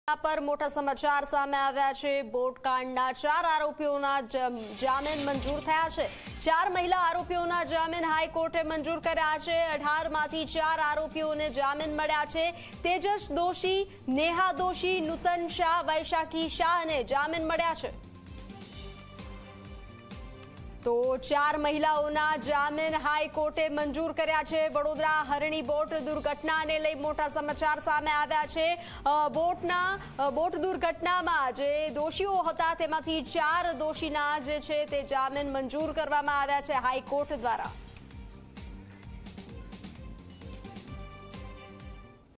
Court approves remand of four women in Vadodara's Harni lake boat tragedy that claimed 14 lives
વડોદરાના હરણી બોટકાંડ પર મોટા સમાચાર---

બોટકાંડના ચાર આરોપીના જામીન મંજૂર થયા---

ચાર મહિલા આરોપીઓના જામીન હાઈકોર્ટે મંજૂર કર્યા--

18માંથી ચાર આરોપીને જામીન મળ્યા--

તેજસ દોશી, નેહા દોશી, નૂતન શાહ, વૈશાખી શાહને મળ્યા જામીન